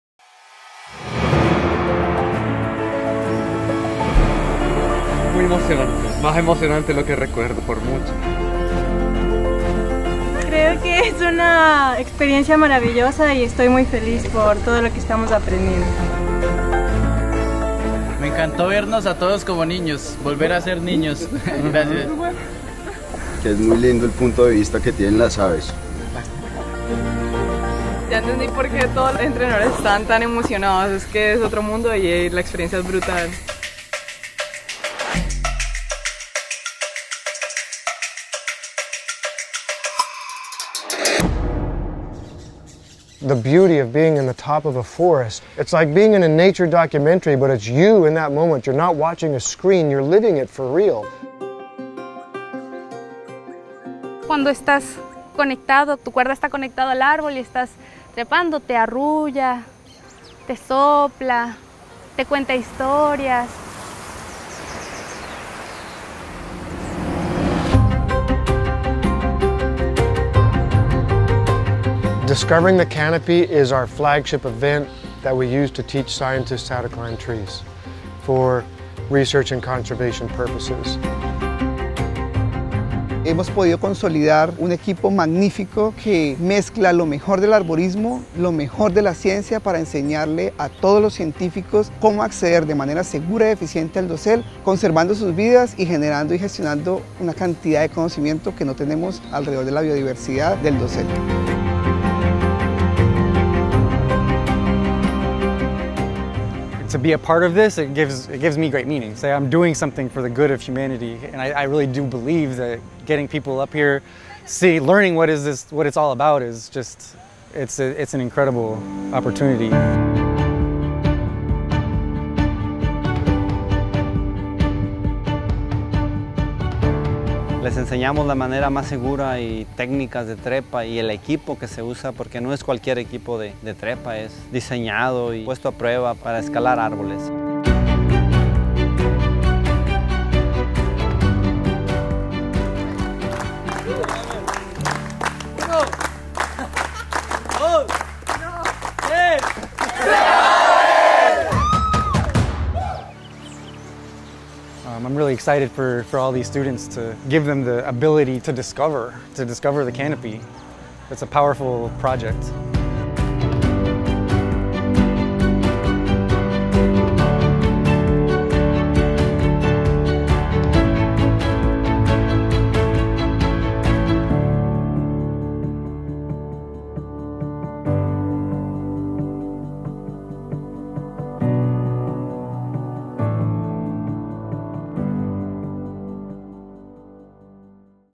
DESCUBRE EL BOSQUE SOBRE EL BOSQUE
¿Quieres descubrir un mundo inexplorado, el bosque que vive sobre el bosque?

Las partes superiores de un bosque conforman su propio ecosistema. El dosel del bosque difiere del interior del bosque en muchas formas. El microclima es más variable, expuesto a vientos, lluvias y luz solar directa. Hay plantas, insectos y animales adaptados a este clima severo y que solo viven en el dosel. Y debido a que pocos científicos conocen métodos para trepar a los árboles más altos, sabemos muy poco sobre la vida en el dosel. En países tropicales en desarrollo donde el dinero, el equipo y la capacitación son difíciles de encontrar, el número de biólogos del dosel es aún menor.

Descubriendo el Dosel está reescribiendo el futuro del descubrimiento, la ciencia y la conservación de la selva tropical. Descubriendo el Dosel es un programa de entrenamiento de élite que equipa a biólogos de América Latina con las habilidades necesarias para trepar a los árboles más altos de la selva tropical. En una semana de entrenamiento, los estudiantes pasan de ser novatos a escaladores avanzados. Con sus nuevas habilidades, regresan a casa para observar, documentar y conservar la biodiversidad en sus países de origen. Nuestro equipo de instructores es excepcional, e incluye inventores, científicos, arboristas profesionales y campeones de escalada en árboles.

Este curso está reservado exclusivamente para profesionales de la ciencia y la conservación de América Latina. El setenta por ciento de los costos del curso son cubiertos por donaciones y patrocinios porque nuestro objetivo es capacitar a futuros científicos del dosel, y el costo del curso es mucho mayor que los modestos recursos de nuestros estudiantes.

Gracias a Kevin Bingham, Noel Rodriguez, Patrick Brandt, Asumi Komatsu y Peter de Vries por ser los increíbles instructores que hicieron de 2024 un evento que cambió la vida de todos nosotros.

Gracias a todos los patrocinadores mencionados al final del video por financiar lo que es una experiencia única en la vida para nuestros estudiantes.

Descubriendo el dosel está organizado por la Fundación Alianza Natural y Canopy Watch International.